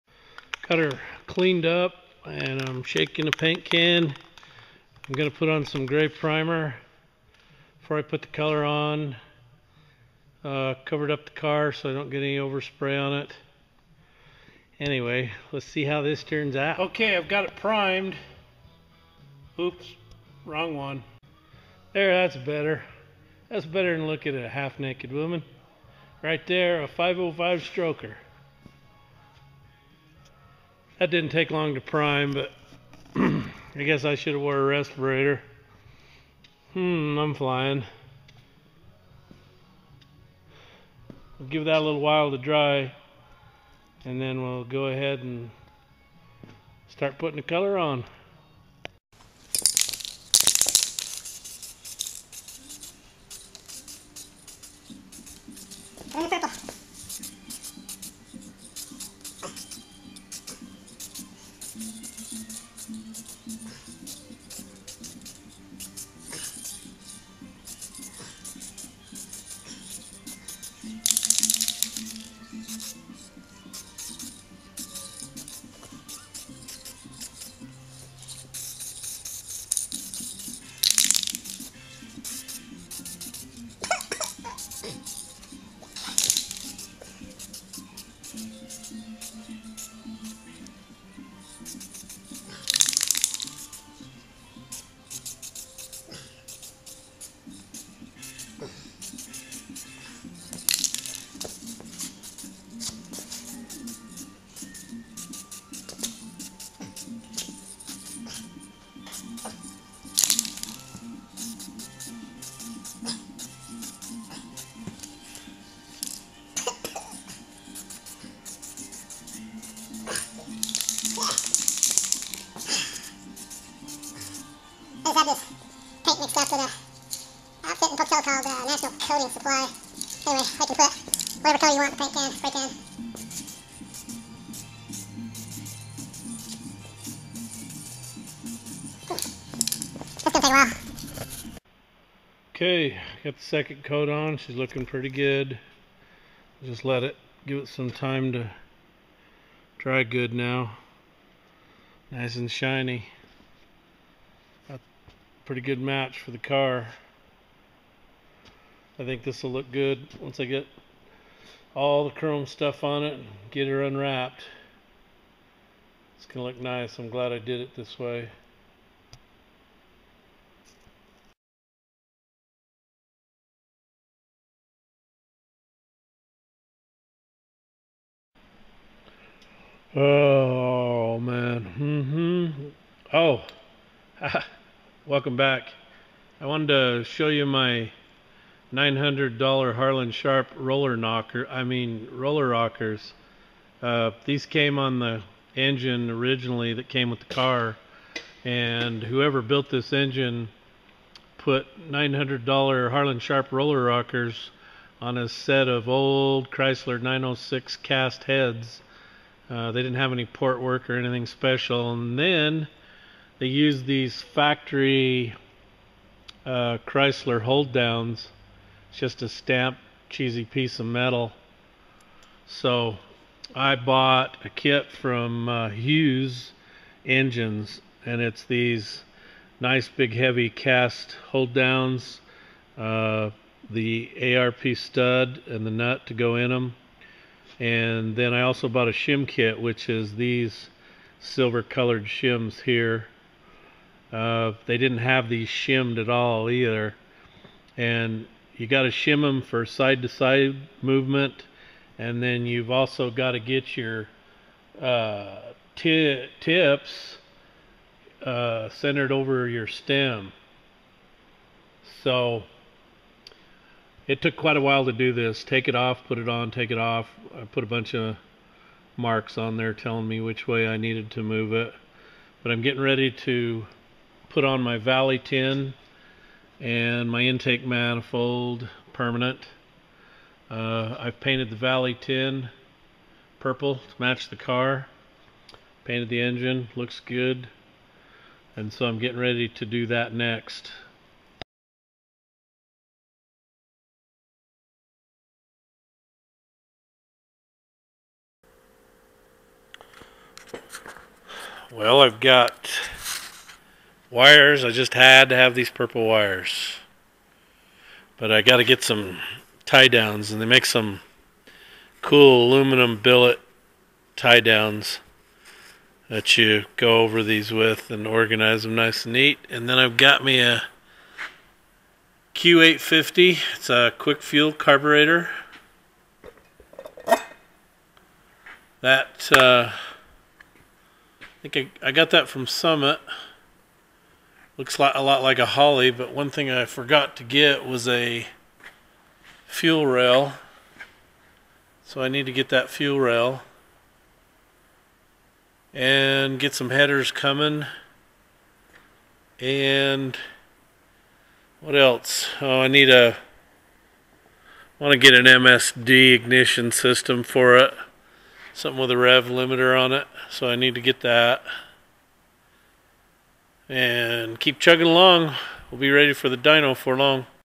Harland Sharp Roller Rockers
The 505 big block Chrysler is almost ready to go to the dyno